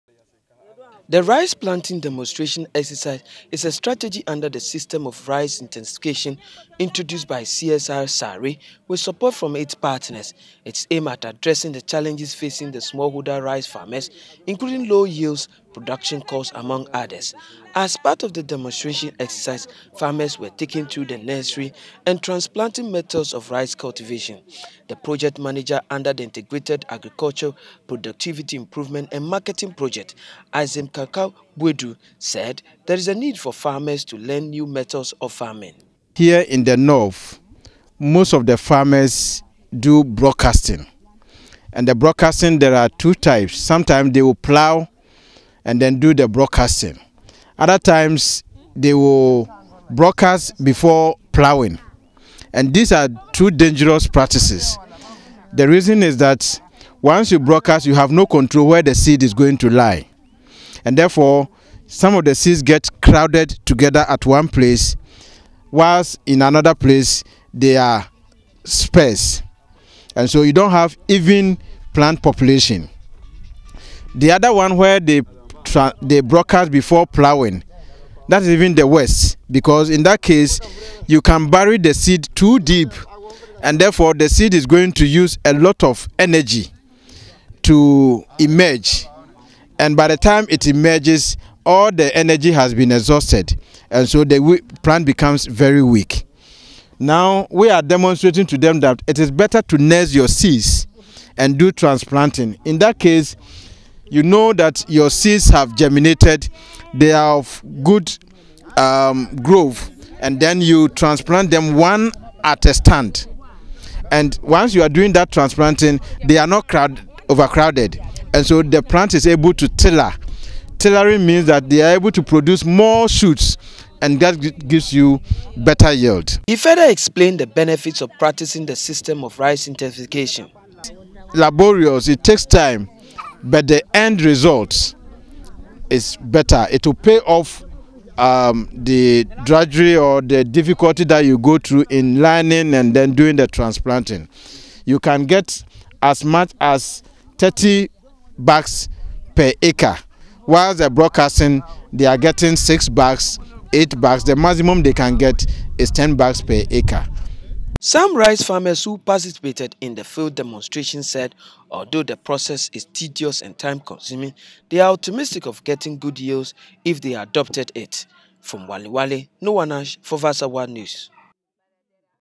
Rice Farmers Urged to adopt Nursing and Transplanting methods for Rice Cultivation-INTAPIMP 15
Small holder farmers in rice production in the northern region have been advised to adopt nursing and transplanting of rice cultivation to avoid over crowding and also increase crop yield on their farms. The Project Manager under Integrated Agricultural Productivity Improvement and Marketing Project, Isaac Kamkam Boadu,said this in an interview during a rice planting demonstration in the West Mamprusi District.

The rice planting demonstration exercise is a strategy under the System of Rice Intensification (SRI), introduced by the CSIR-SARI with support from its partners.

It is aimed at addressing the challenges facing the smallholder rice farmers including low yields, production cost among others.
As part of the demonstration exercise, the farmers were taken through the nursing and transplanting method of rice cultivation.

The Project Manager under Integrated Agricultural Productivity Improvement and Marketing Project, Isaac Kankam Boadu, said there is the need for farmers to learn new methods of farming.

He further explained the benefits of practicing the System of Rice Intensification (SRI).

Some rice farmers who participated in the field demonstration said although the process is tedious and time consuming, they were optimistic of getting good yields if they adopted it.

From Walewale, Noah Nash for Viasat 1 News